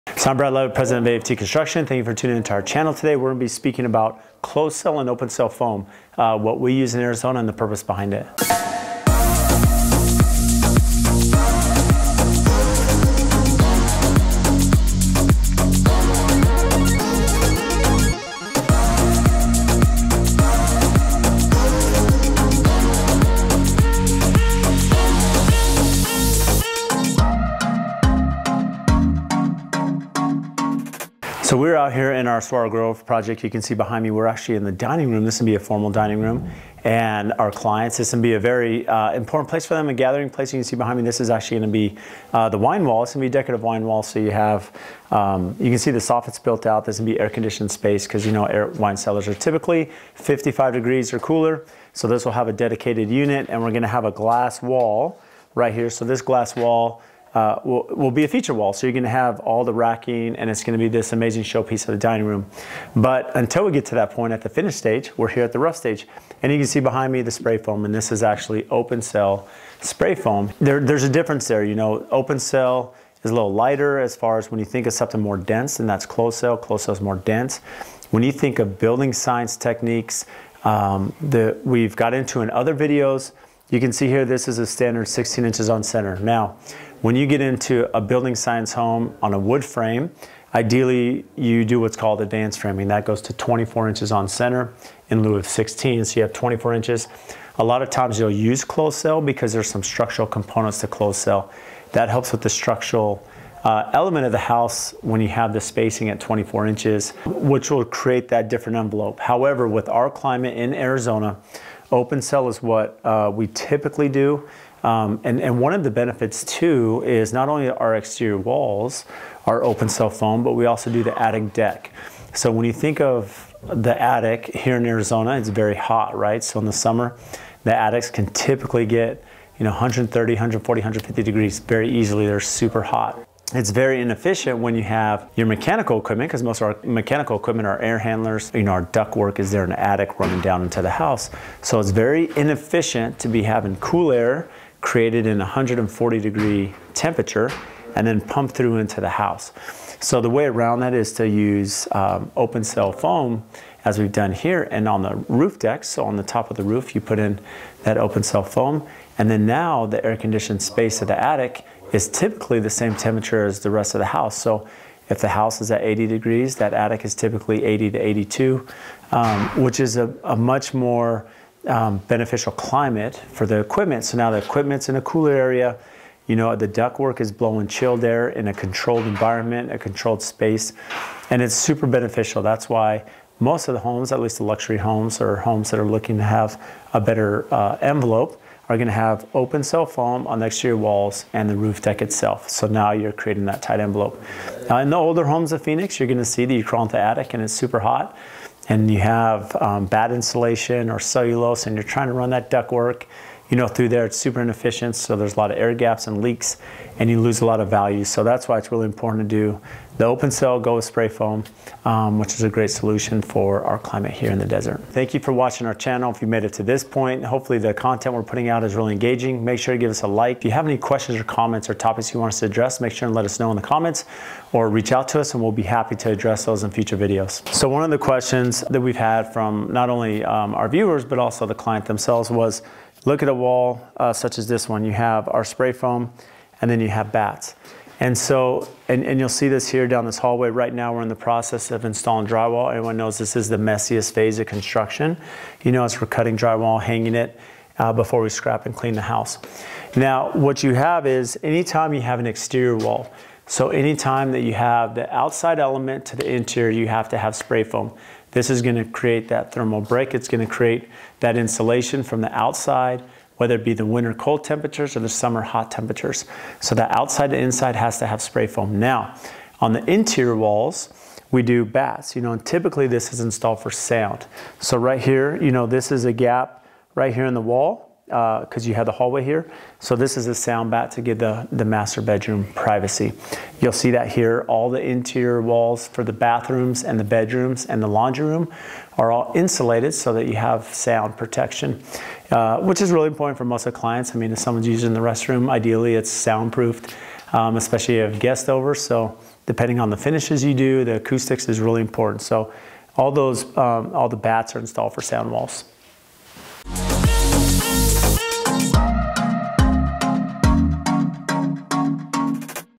Open Cell vs. Closed Cell Spray Foam Insulation [4K] | Which One Does AFT Use Most?
Open Cell or Closed Cell Spray Foam Insulation? In this episode, Brad discusses open cell and closed cell insulation. And why in Arizona you see more open-cell foam usage.

Camera Gear:
Canon EOS R
DJI Ronin SC
MeFoto GlobeTrotter Tripod
Rode VideoMicro Mic
Rode SmartLav+
Zoom H1N Recorder
Rode Wireless Go